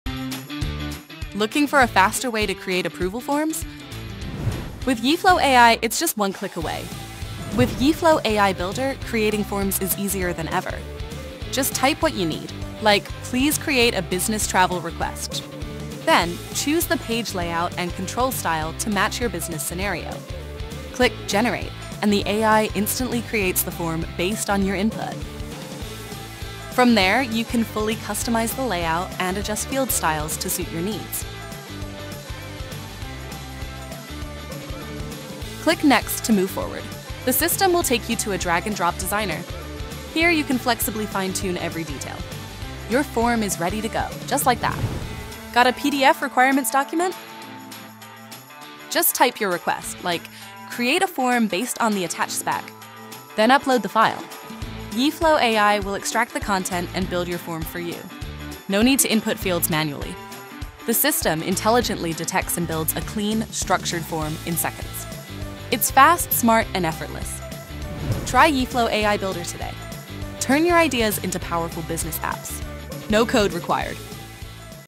Build approval forms with AI Builder | Yeeflow
Say hello to a smarter, faster way to build approval forms — with Yeeflow AI Builder.

In this video, we introduce how Yeeflow’s AI Builder helps you generate approval forms instantly using natural language. No coding, no complexity — just tell the AI what you need, and Yeeflow takes care of the rest.

Whether you’re setting up approval workflows for leave requests, purchase orders, expense claims, or IT service requests, AI Builder simplifies the entire process.

Yeeflow is an enterprise-grade no-code platform designed for building custom applications, automating workflows, and managing data — and now, with AI Builder, it’s even easier to get started.

🎯 In this video, you’ll learn:

• What Yeeflow AI Builder is and how it works
• How to generate a fully functional approval form in seconds
• How to continue customizing your form with Yeeflow’s visual designer
• Use cases across HR, Finance, IT, Procurement, and more

AI Builder is part of Yeeflow’s vision to bring the power of AI and no-code together — so you can build smarter business applications faster.